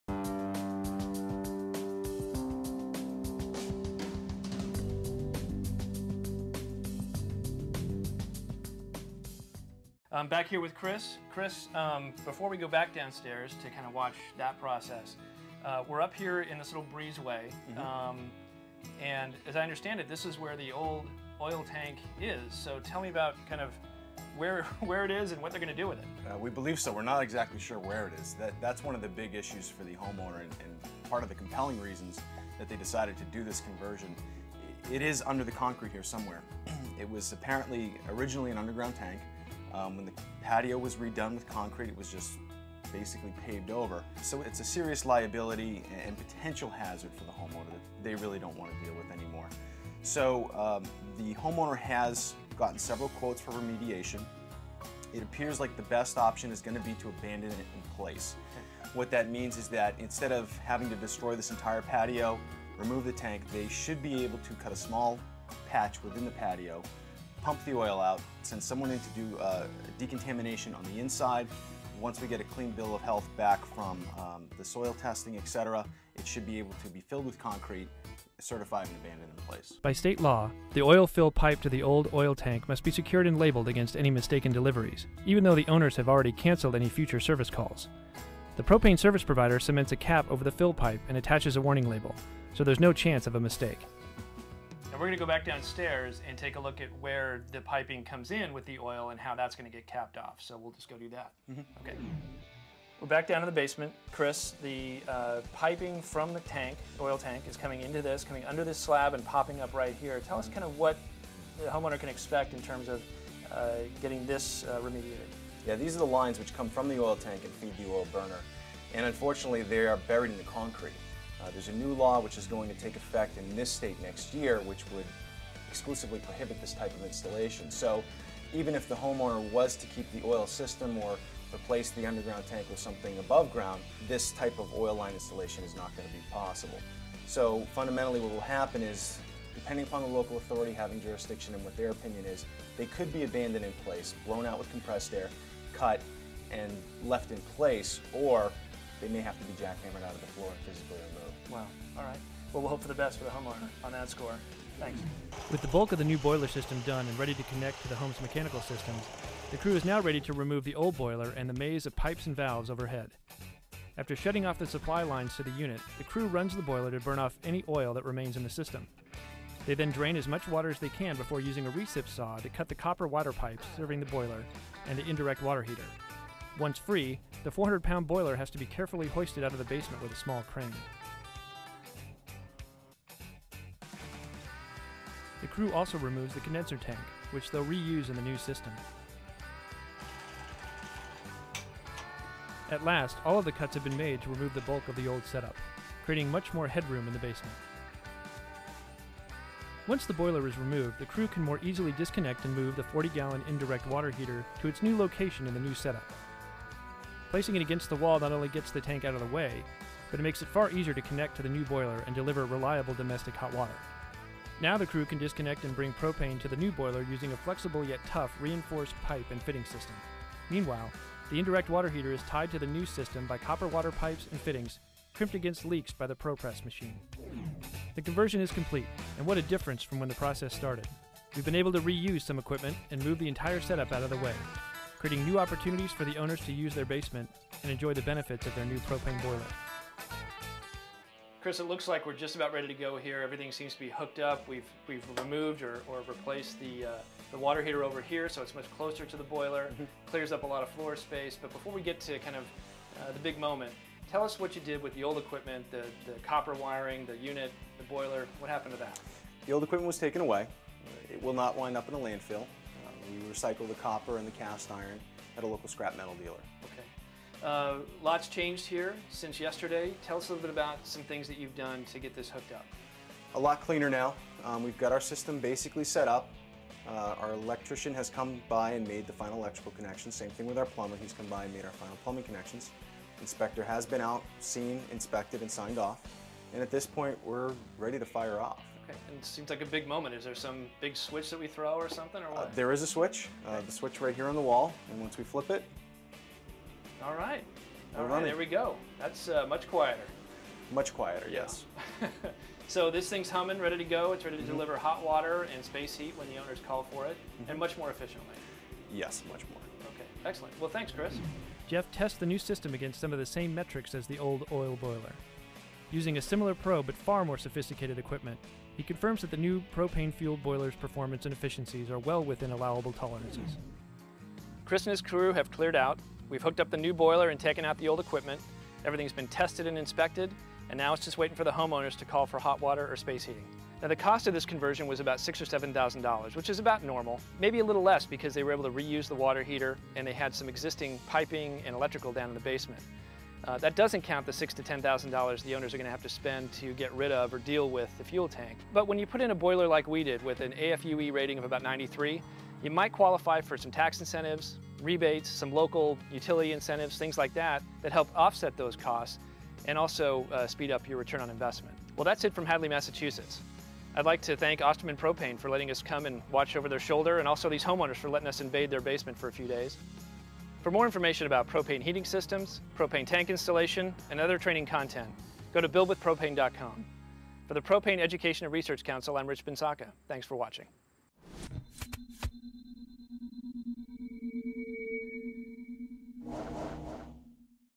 Converting an Oil-Fueled Boiler System to Propane, Part 2
Part 2 of 2: This video takes viewers through the steps of converting an oil-fueled boiler system to one based on a high-efficiency propane boiler. Such a retrofit is becoming popular in many states, especially in the New England region, among homeowners looking for energy savings and lowered carbon emissions.